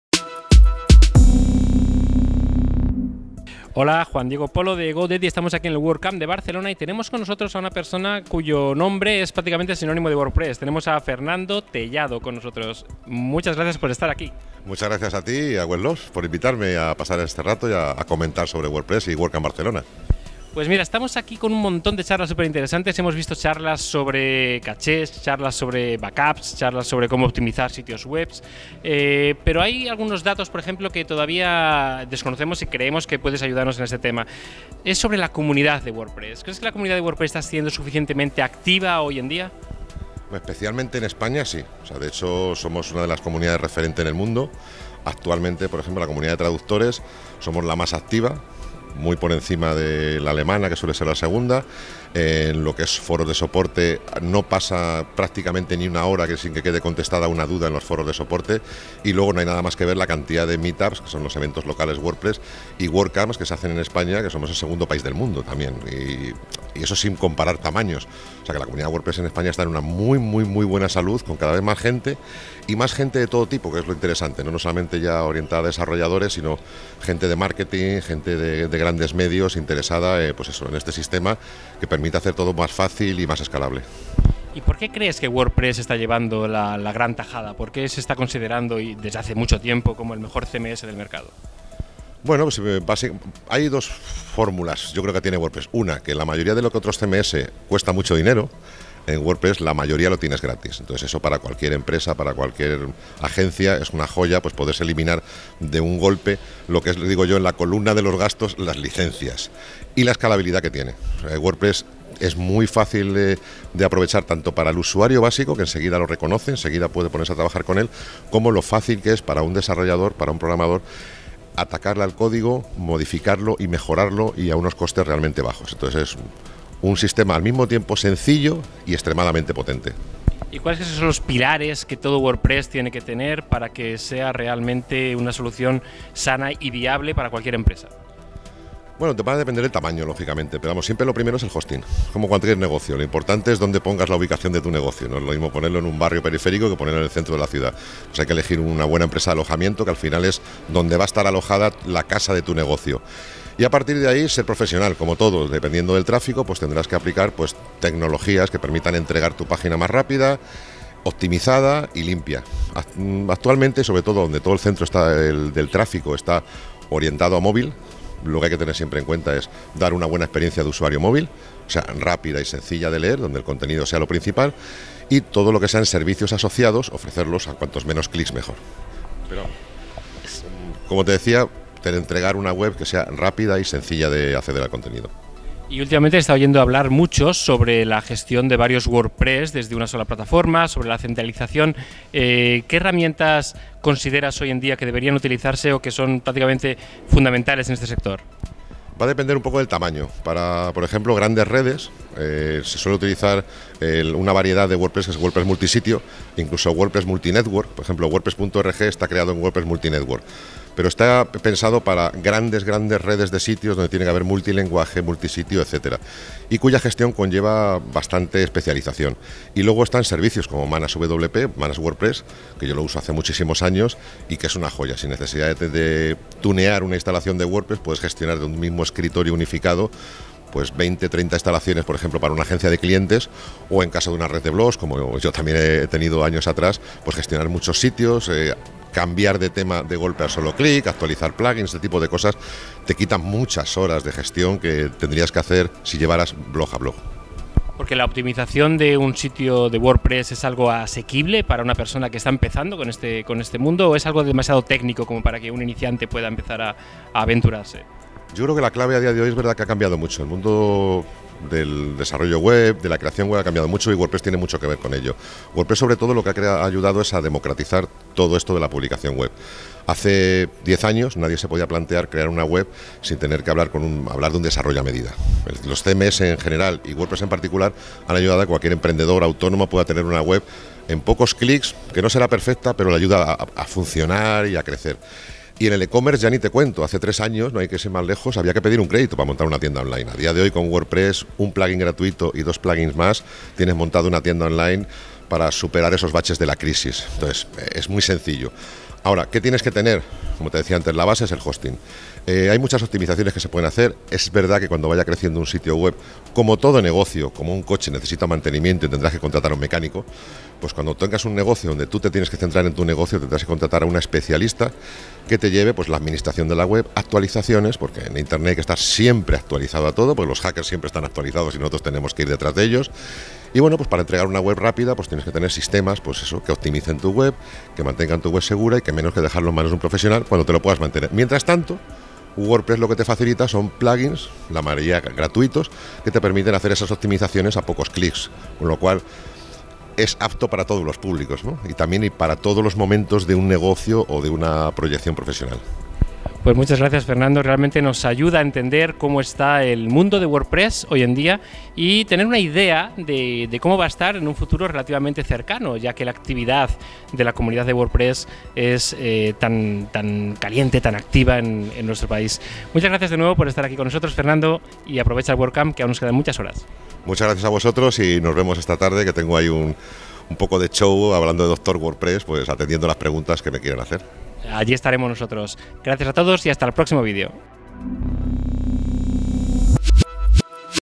Entrevista a Fernando Tellado en WordCamp Barcelona
Hablamos en la WordCamp Barcelona 2018 con Fernando Tellado, que asistió como ponente para la sesión de preguntas de 'Doctor WordPress', y que nos ha contestado en esta entrevista a preguntas sobre la comunidad WordPress en España, las claves que hacen de WordPress el principal CMS y las herramientas imprescindibles para trabajar con WordPress, entre las que destaca ManageWP que utiliza desde hace años para gestionar y mantener sus webs.